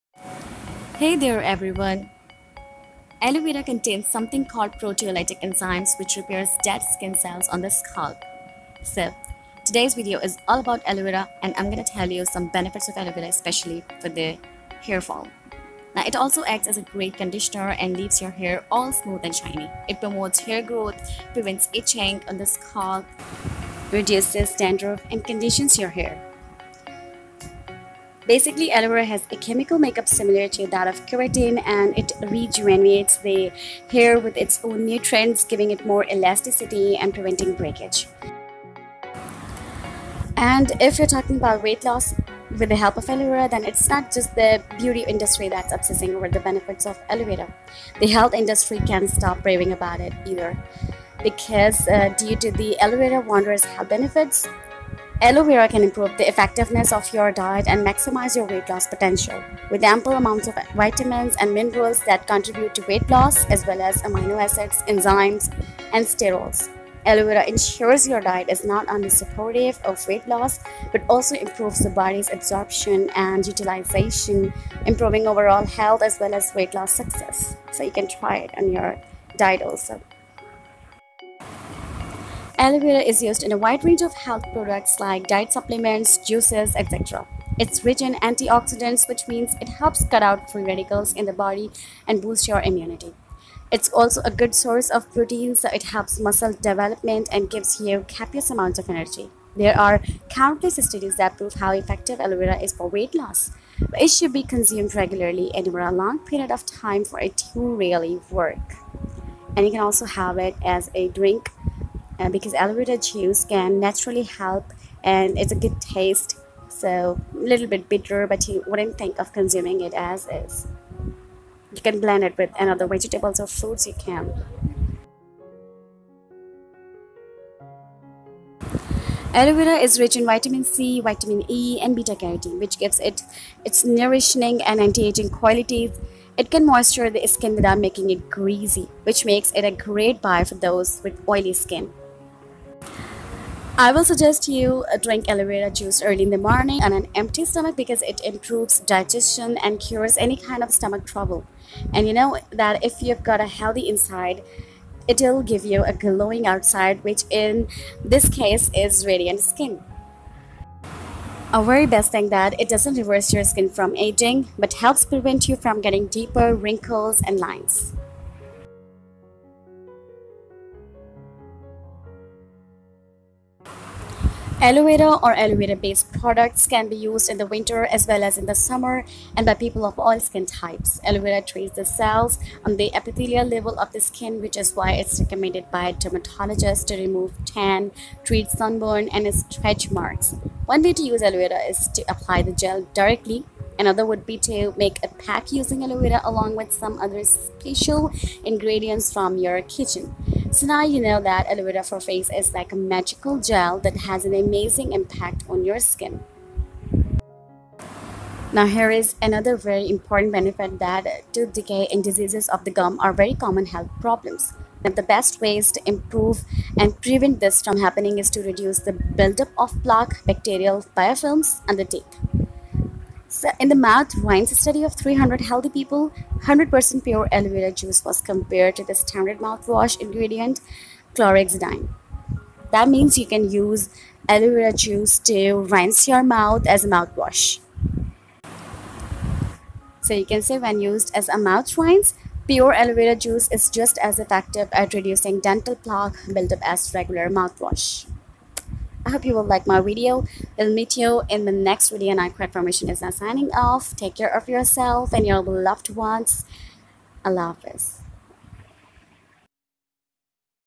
ALOE VERA WITH MULTIPLE BENEFITS | ALOE VERA GEL FOR SKIN, HAIR & WEIGHT LOSS
aloe vera can be used in winter and summer both, its effects will always be pure for skin , hair, and many more.

Aloe vera is used in a wide range of health products, like diet supplements, juices etc. It's rich in anti-oxidants which means it helps cut out free radicals in the body and boost your immunity. It's also a good source of protein so it helps muscle development and gives you copious amounts of energy.

You can find a written copy of this content on my blog at Wix.com/mysite